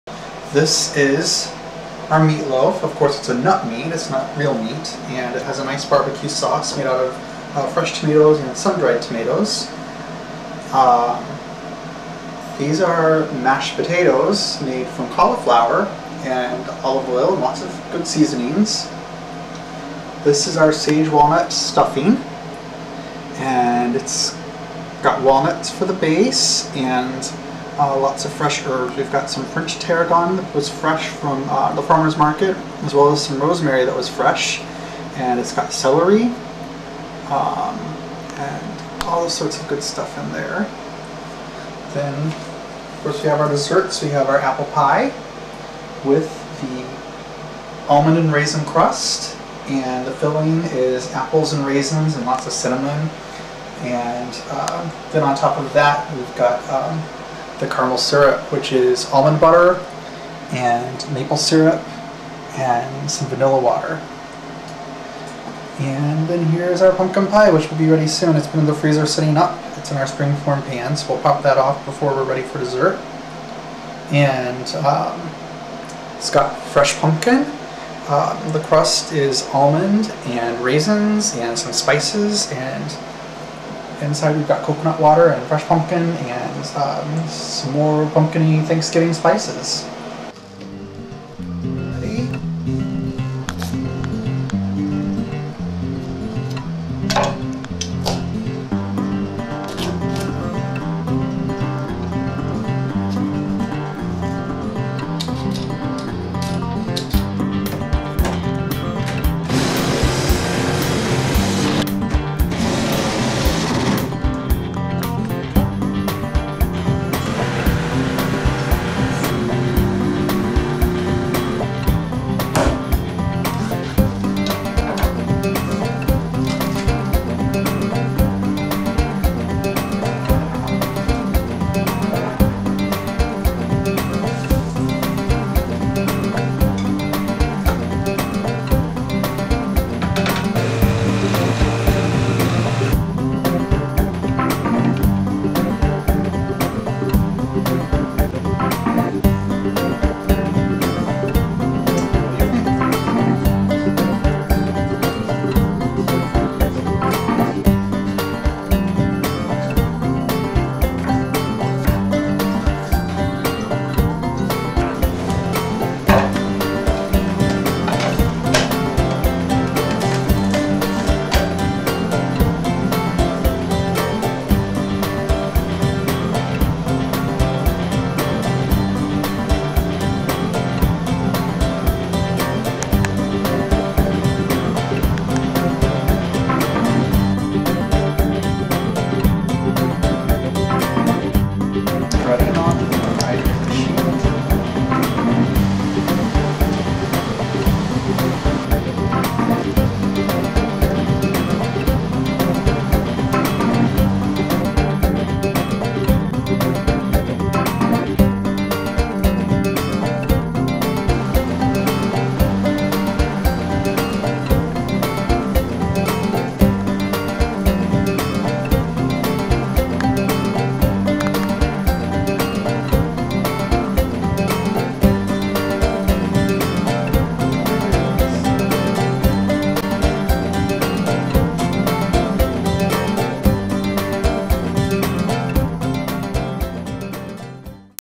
Raw Thanksgiving
Rawvive Reboot Coach Josh spends some time in the kitchen preparing his Raw Thanksgiving feast.

("Meat Loaf" from Juliano's RAW, Mashed "Potatoes" and Savory Herb Stuffing from Matt Amsden's RAWvolution, Caramel Apple Pie from rawon10.blogspot.com, and Pumpkin Pie from Ta-Raw.com.)